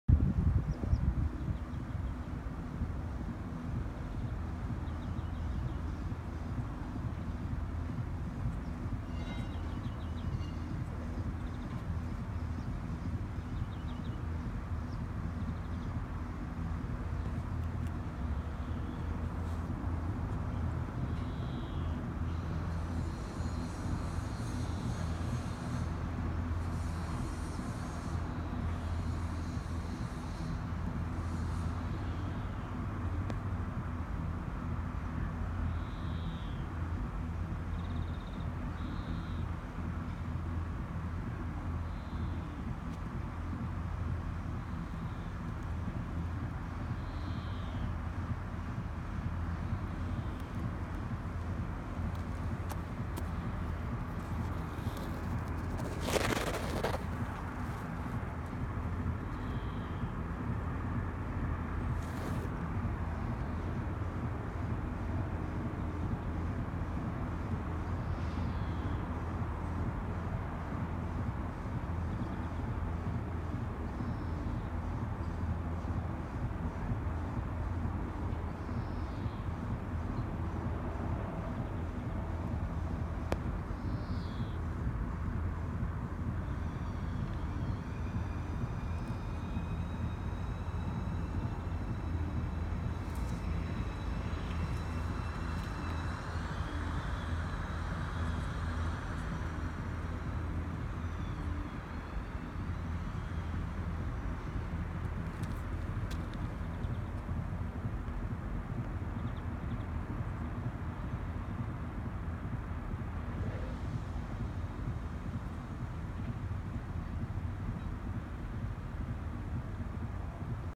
Slaapmutsje (Eschscholzia californica) - 2019-06-27
Het slaapmutsje (Eschscholzia californica, Engels: California poppy), ook wel goudpapaver genoemd, is een kruidachtige plant uit de papaverfamilie (Papaveraceae).